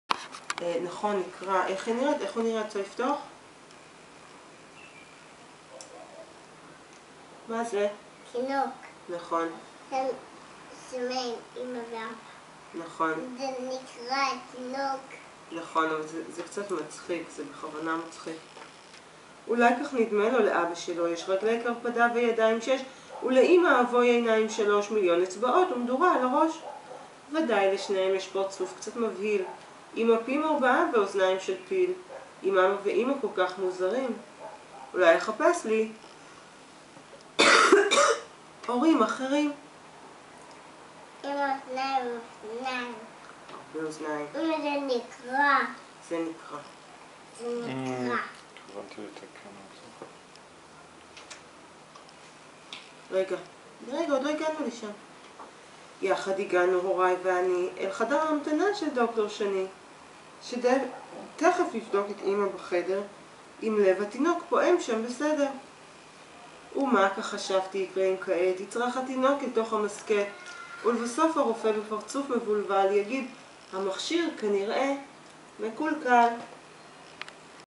Neta reads a story to Shani (Hebrew)
Neta Harel reads a story to Shani Harel Lowenstein in our living room (Israel, in Hebrew)